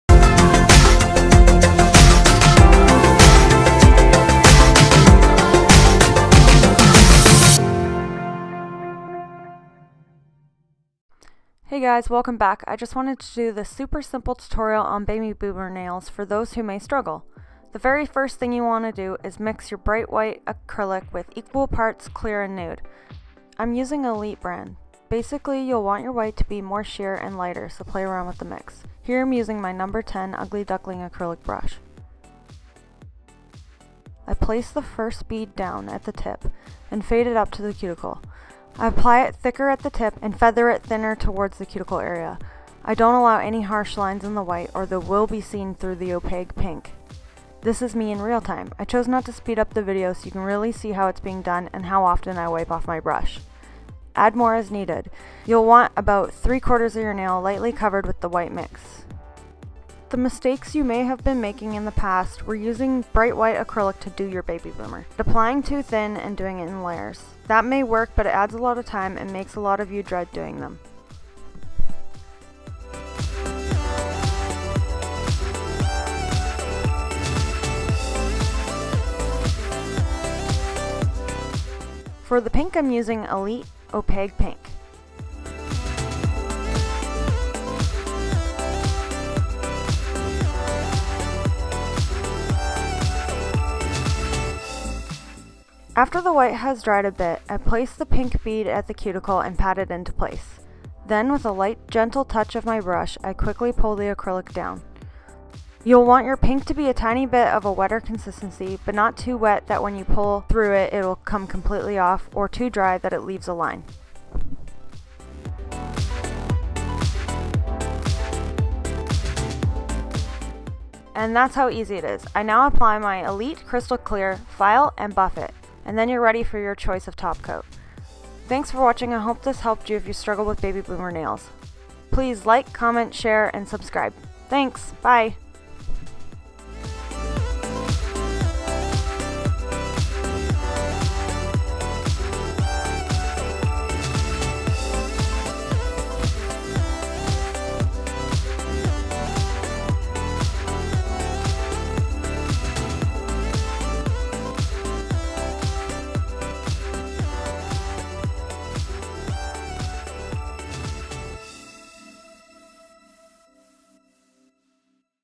THE EASIEST BABY BOOMER NAIL TUTORIAL | Elite beauty supplies
Todays quick tutorial is the easiest way to do baby boomer / ombre french faded acrylic nails
if you like videos like this let me know!

PRODUCTS IN VIDEO:
UGLY DUCKLING NAILS: #10 brush

ELITE BEAUTY SUPPLIES: bright white, crystal clear & opaque pink

Some products I feature are sent to me for consideration but all opinions are my own.